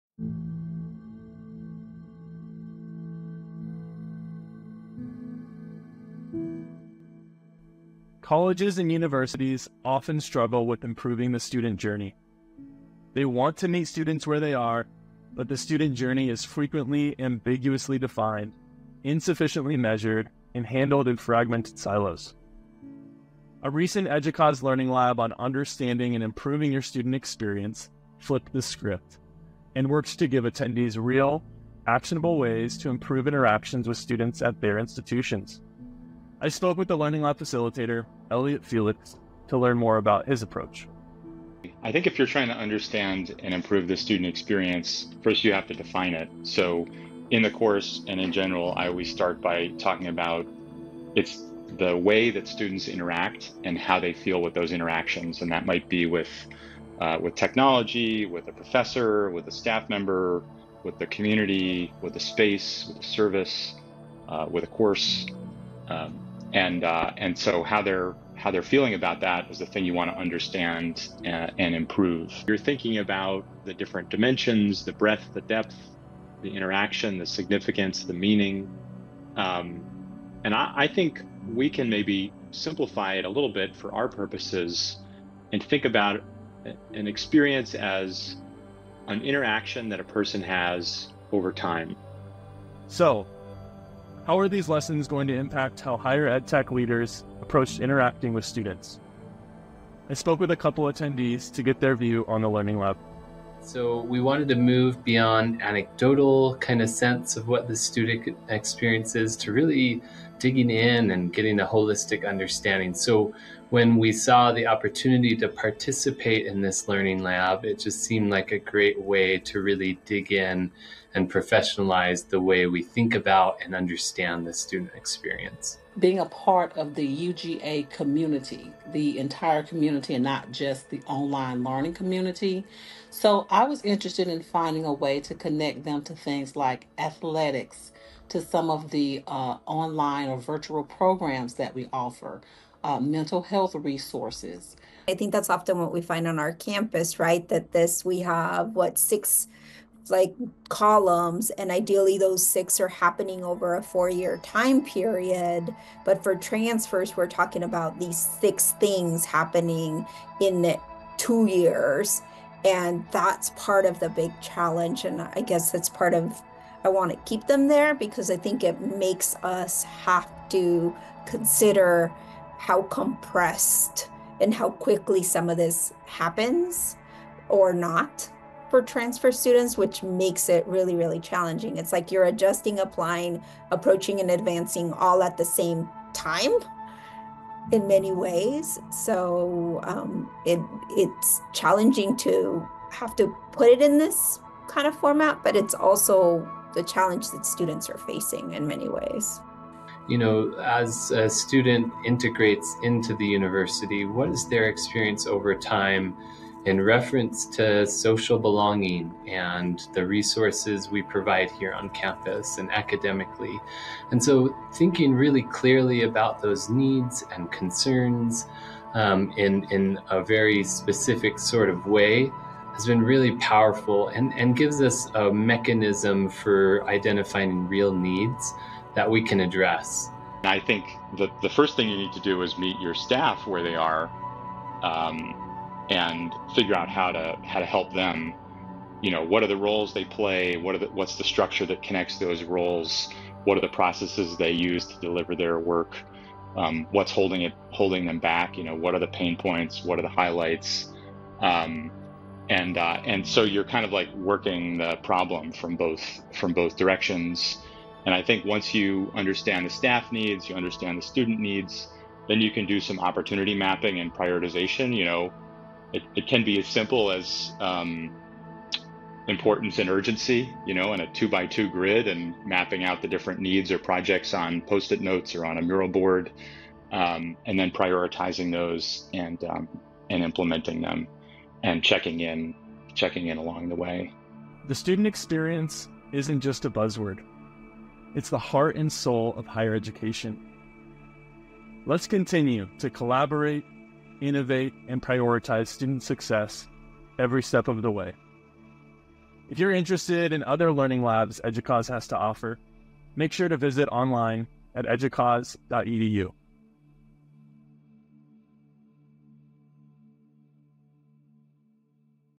Designing Student Experiences – Highlights from an EDUCAUSE Learning Lab
Ben Bongers, EDUCAUSE Professional Learning Manager, talks with Elliot Felix, facilitator of the Understanding and Improving Your Student Experience Learning Lab. You'll hear from Elliott, and learners Justin Paulsen and Sharon Shannon,  to explore how intentional student experience work can help an institution meet students where they are.

Interested in connecting with Elliot Felix? Explore this article for more information on his approach to understanding and improving the student experience